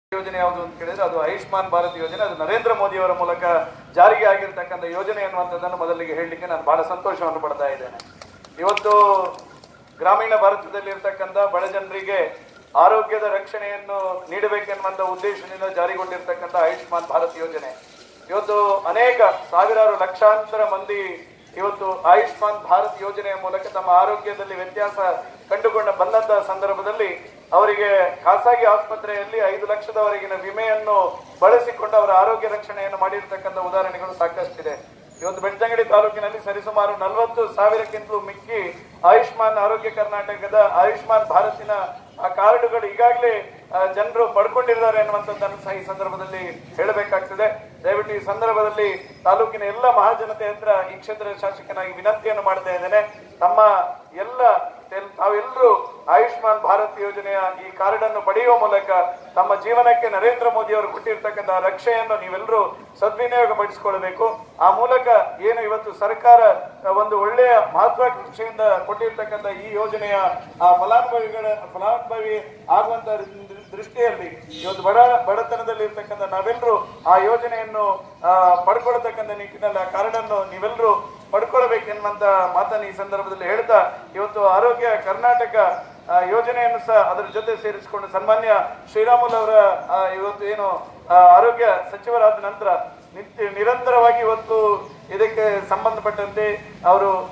ಆಯುಷ್ಮಾನ್ ಯೋಜನೆ ಜಾಗೃತಿ ಜಾಥಾ
ಬೆಳ್ತಂಗಡಿ  ಮಿನಿ ವಿಧಾನ ಸೌಧದ ಎದುರು ಶಾಸಕ ಹರೀಶ್ ಪೂಂಜರಿಂದ  ಜಾಥಾಕೆ  ಚಾಲನೆ